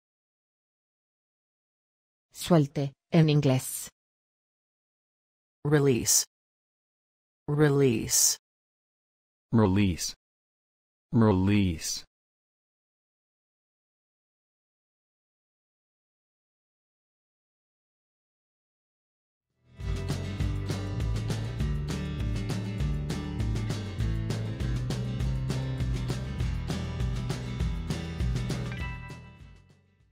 Cómo se dice suelte en inglés
¿Cómo se dice correctamente suelte en inglés? Escuche con nosotros.
¿Cuál es la traducción correcta de la palabra suelte del español al inglés? Habla como los americanos. Además, ¿cómo escribir correctamente la palabra release?

¡Gracias!